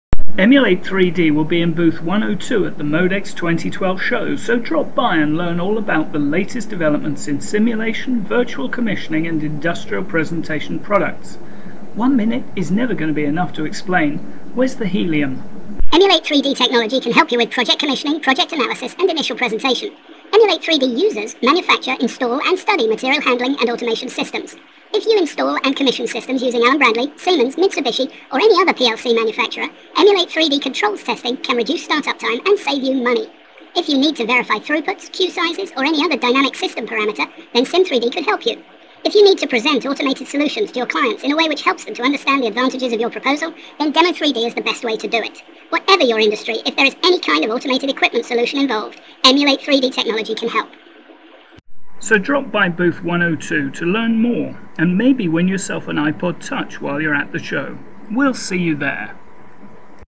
Emulate3D at MODEX 2012
This is a one minute video (a long elevator pitch, if you prefer) to explain to attendees why they should drop by booth 102 to see all the latest in the virtual commissioning of automated systems, throughput simulation, and presentation and system prototyping tools. Look around the show for Emulate3D users - they will be offering the chance to win an iPod Touch as well as showing off some of their great Demo3D videos.

We look forward to seeing you there, but if you can't wait, go now to learn more.